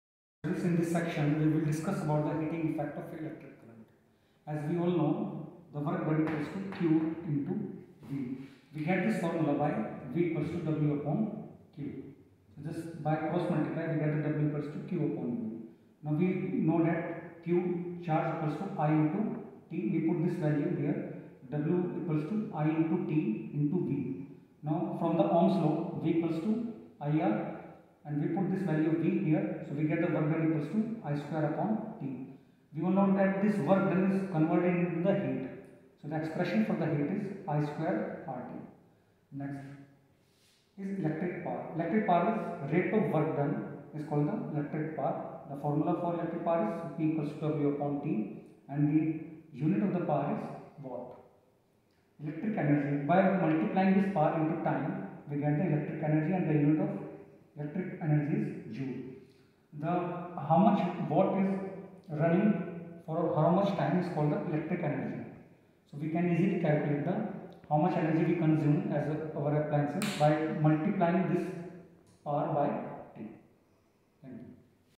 Class- X- Science (Physics)- Ch.-12- Electricity- Part-1
Lectured by Mr. Raman Kumar (Teacher, Science), Maples Academy, Deoband, Distt.- Saharanpur (U.P)